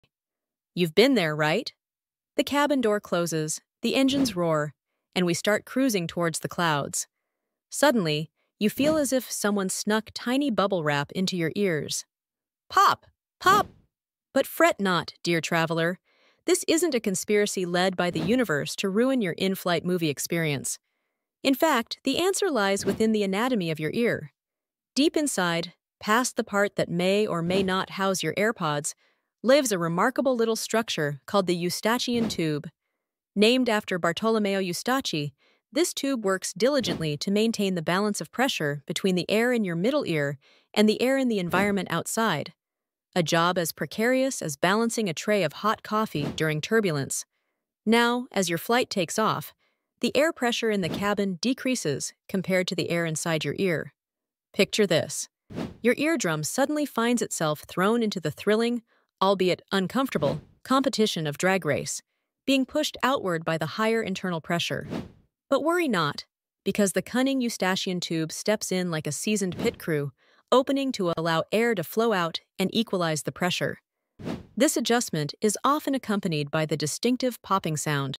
Why Your Ears Pop When You Fly
Air travel can be exhilarating, but that ear-popping annoyance? Not so much! Discover the fascinating role of the Eustachian tube as it balances the pressure in your ears with the changing atmosphere of a flight. Say goodbye to discomfort and hello to understanding! This little marvel of anatomy ensures your eardrums don't face the pressure alone, adjusting like a pro pit crew during takeoff. Learn how this intricate process works and why you hear that distinctive pop when you ascend. Your next flight will never feel the same again!